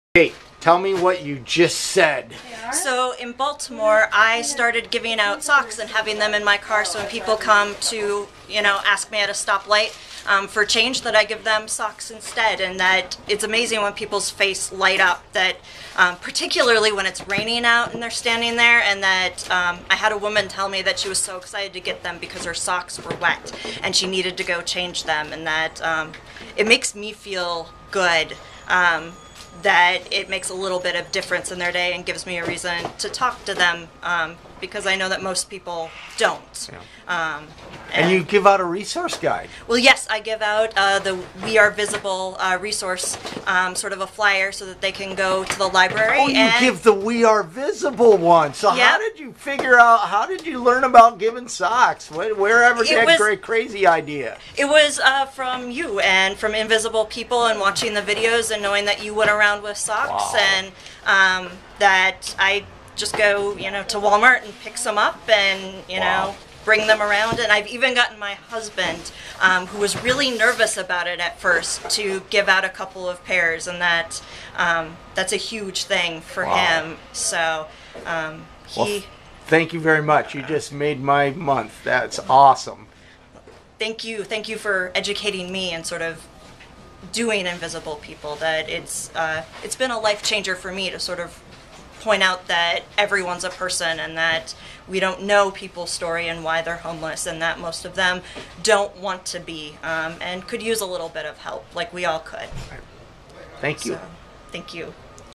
Woman Gives Socks Because Of InvisiblePeople.tv
This year's Nonprofit Technology Conference's Day of Service helped Washington DC's homeless services and our environment. I love that this conference gives back to the community in such a huge way.